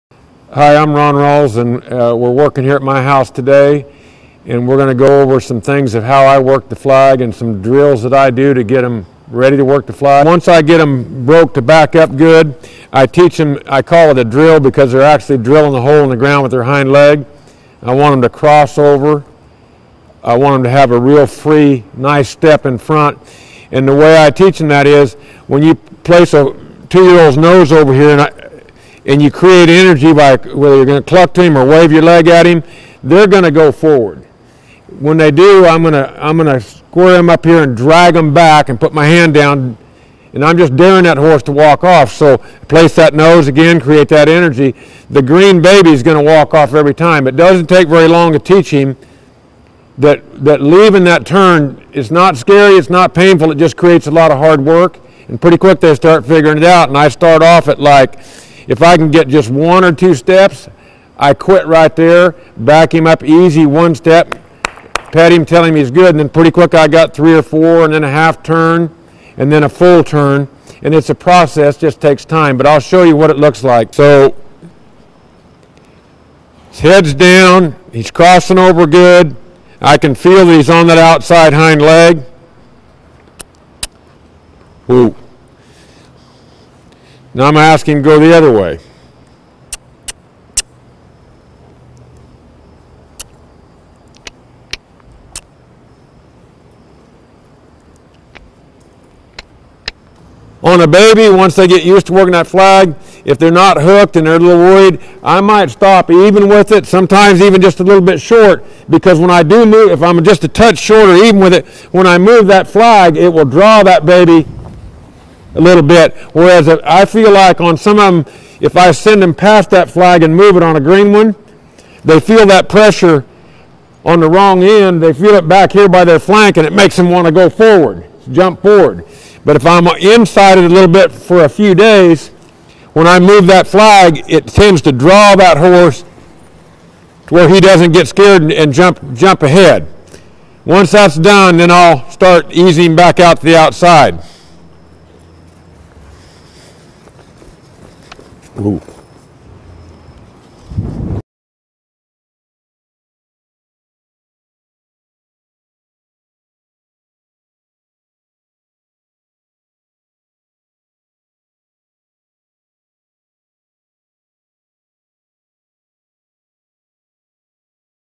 How To Improve Your Turn-Around For The Cow-Horse Events- Ron Ralls
Two-times Worlds Greatest Horseman, Ron Ralls shows you a great exercise to improve your turn and how to work the flag correctly.
Ron shows you a great exercise to improve your turn and how to work the flag explaining:
*Which foot is the pivot foot and why.
*How to use your in-direct rein when teaching this exercise.
*Why you don’t want them stepping behind with the front feet for the cow-horse events.
*How to get a green horse to accept the flag.
*How to create more draw by staying inside the flag.
*What to do if your horse over rotates the turn.
*Why a horse will turn with the shoulder first and how to correct it.
Plus loads more